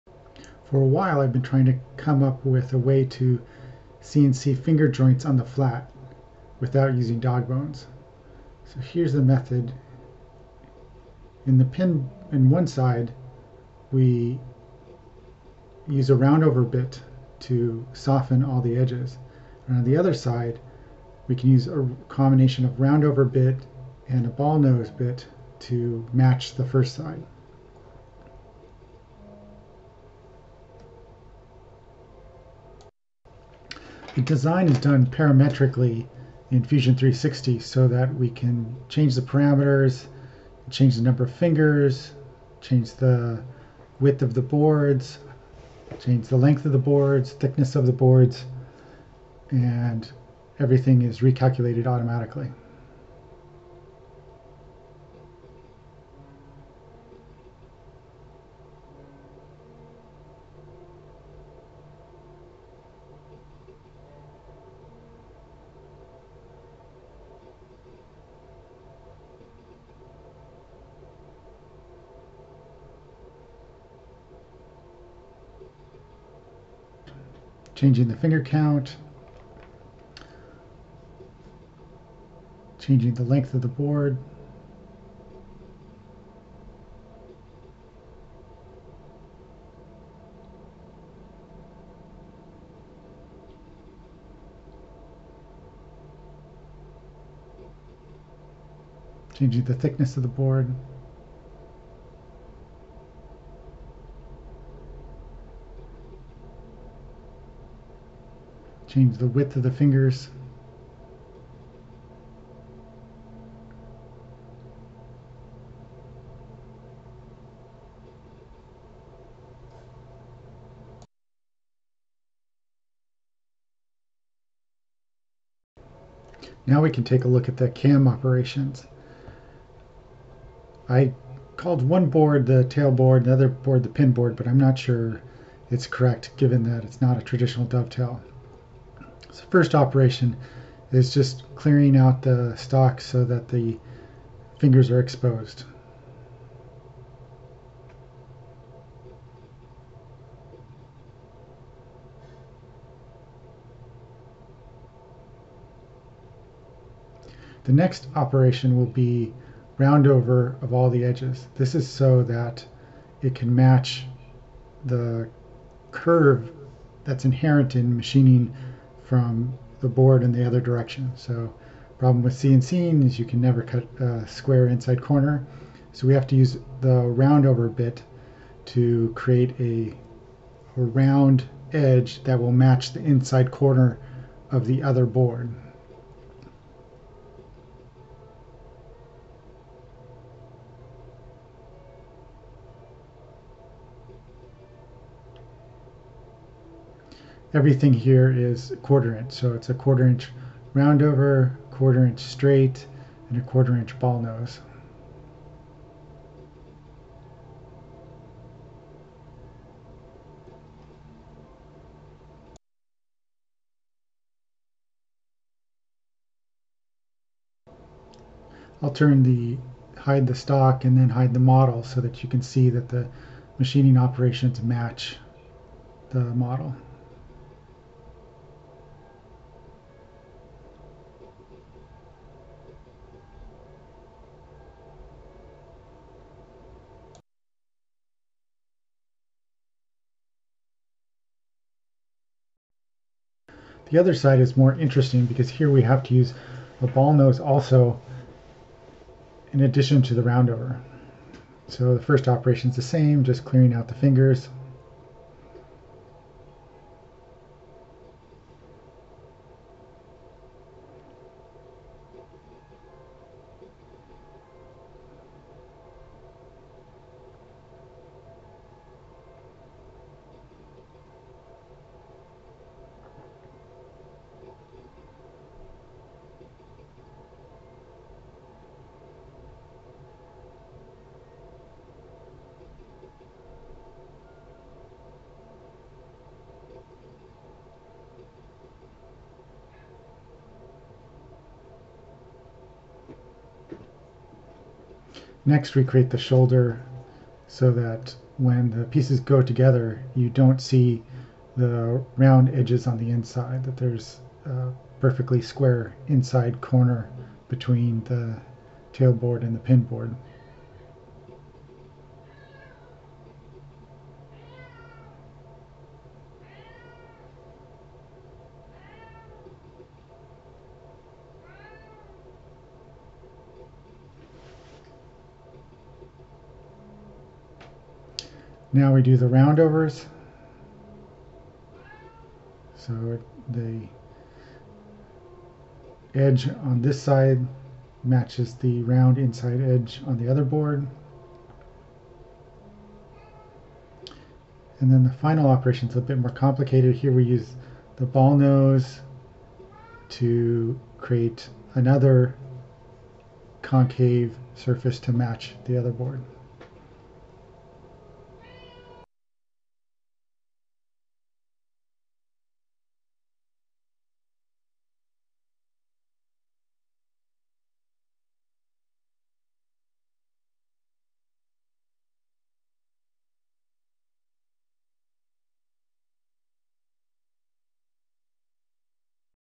CNC finger joints on the flat
I was trying to come up with a way to CNC finger joints one sided for efficient production. This method requires three bits, straight, roundover and ball nose. However, it seem fairly efficient. Also the joint is fully engaged. The entire surface of each side of the joint is in contact with the other side.

Partial video of the cnc cutting the design.